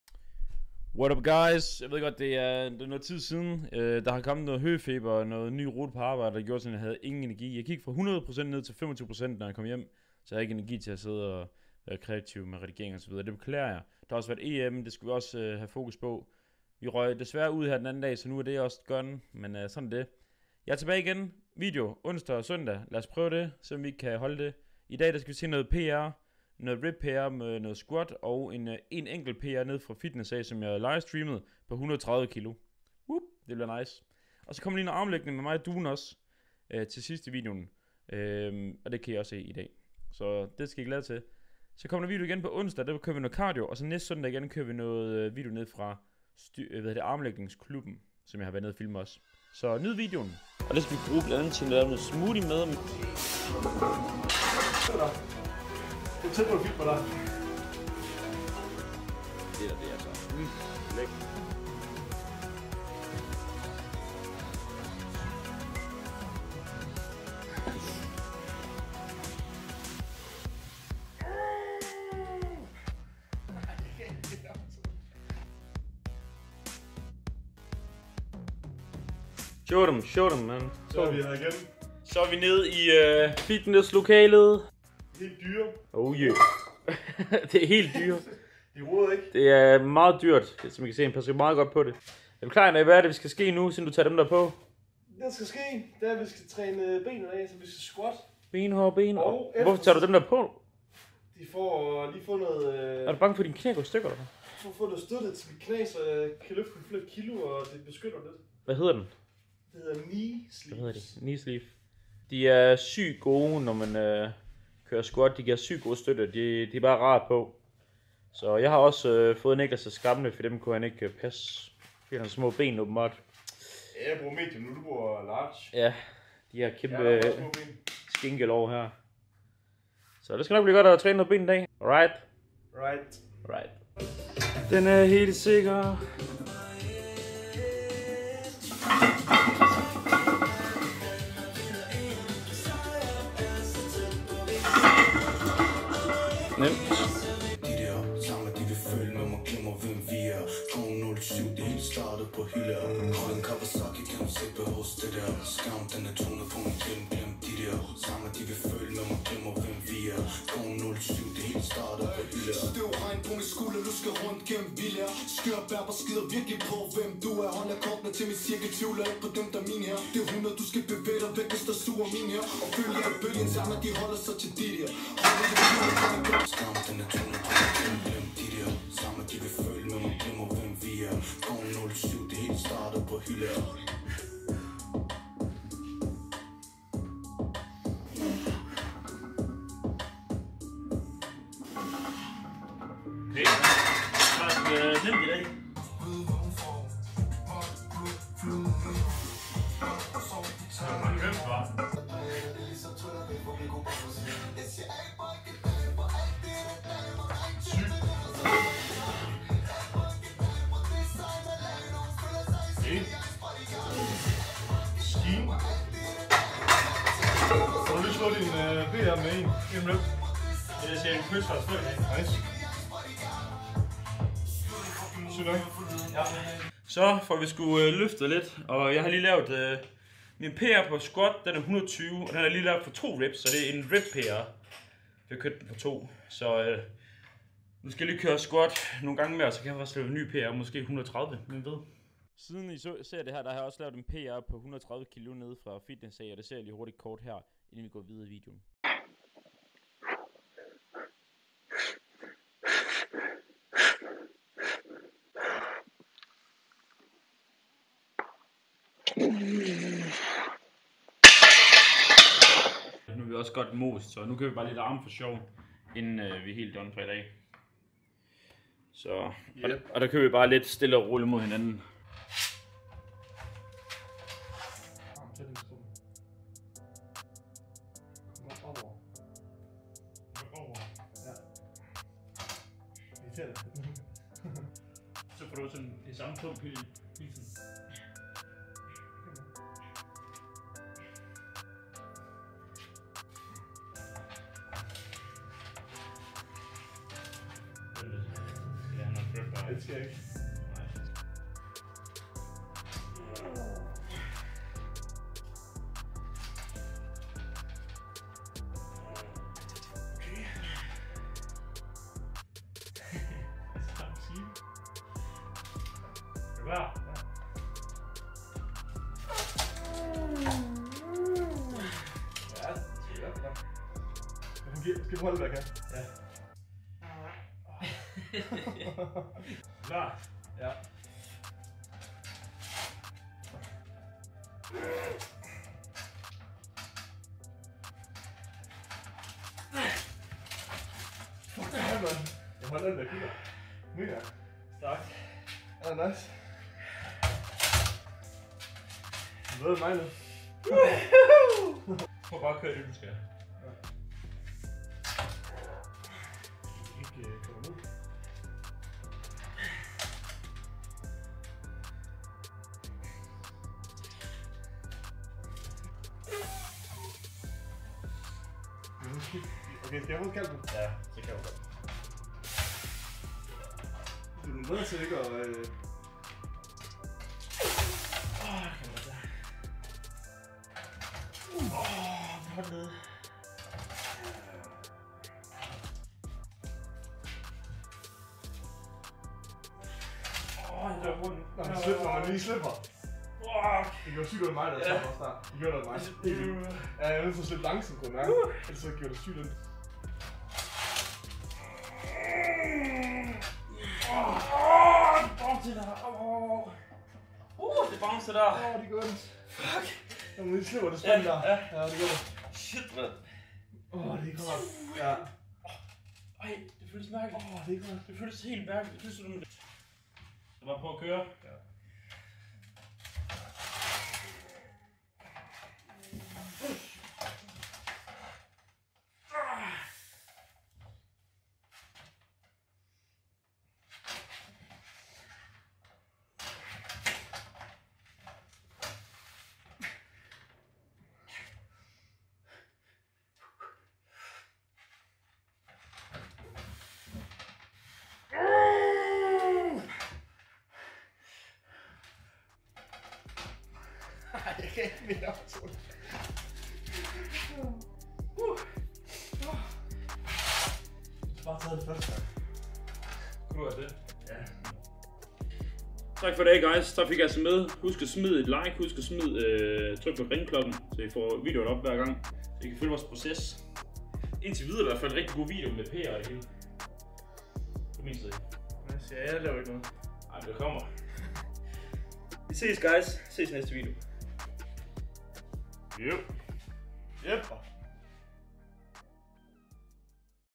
Kampen Mod Et Sundere Liv! - Ny Squat PR! + Armlægning (Afsnit 4)
Kampen Mod Et Sundere Liv er en serie, hvor jeg kæmper for at få en sundere livstil, samt alt snak om træning, kost, og mindset.

I dette afsnit træner vi ben, og jeg formår at lave et nyt squat PR. Det vil sige, at jeg laver et løft jeg ikke har kunne gøre før hen. Det løft jeg laver i denne video, er det tungeste løft jeg har lavet i Squat.

Tak fordi i vil følge med!

Tak for støtten! ✌